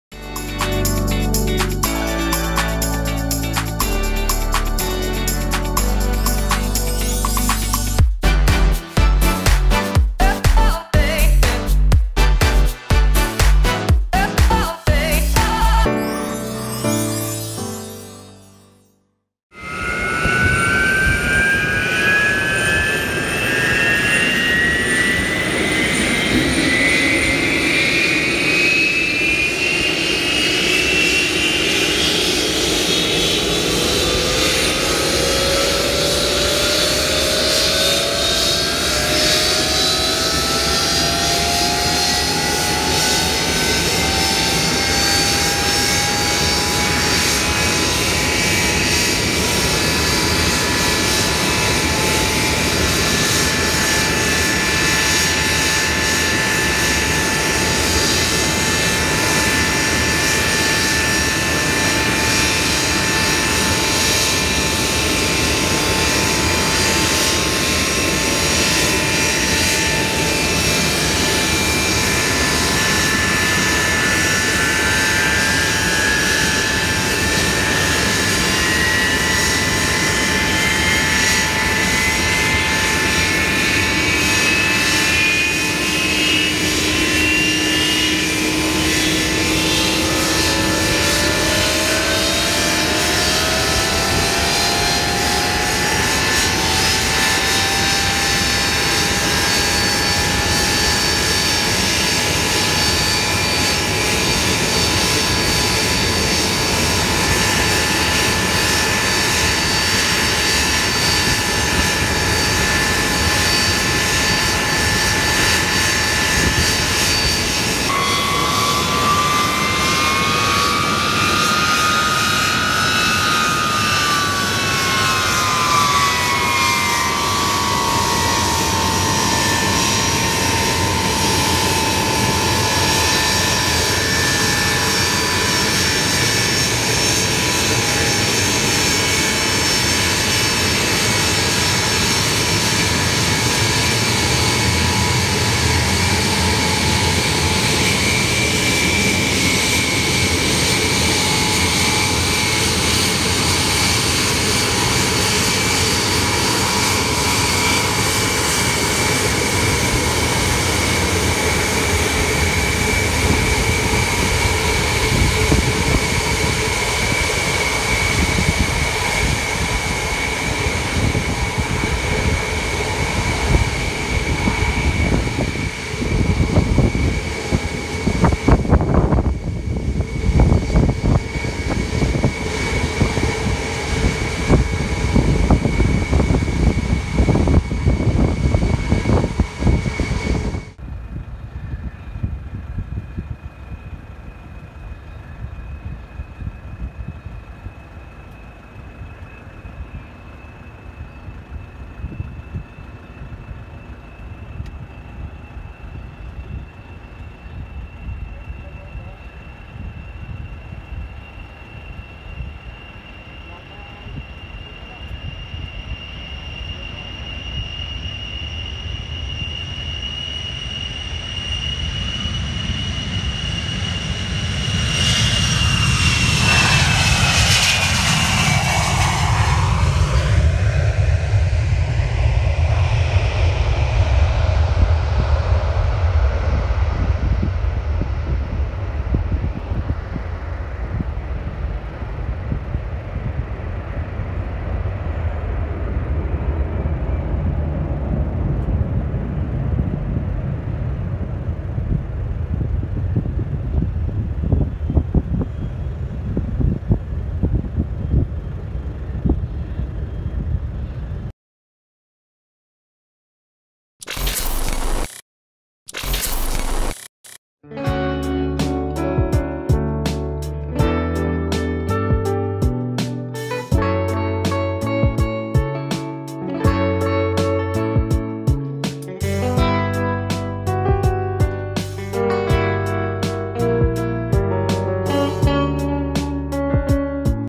ILYUSHIN IL-76 using almost the full Runway for DEPARTURE! Jakarta to Delhi
This Ilyushin IL76 of Ukraine's Fly Sky Airlines operated a special cargo charter flight from Jakarta to Delhi
UR-FSA
Ilyushin Il-76
4 x solovied D-30

Be happy, and safe flight everyone!

twitterr